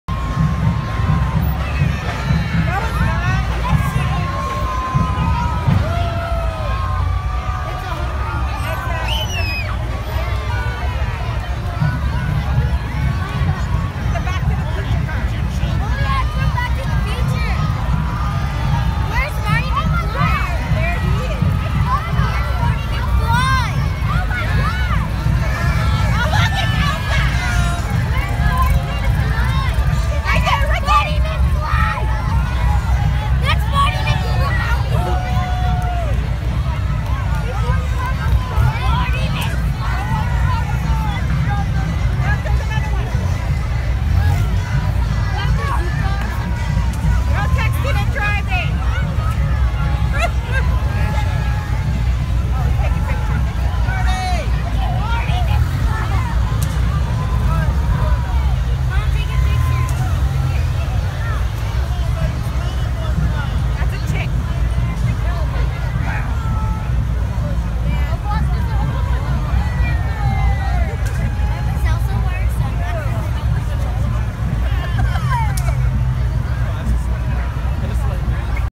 DeLoreans at Portland Starlight Parade 2019
The Pacific Northwest DeLorean Club at the annual Portland Starlight Parade 2019. Always my favorite part of the parade!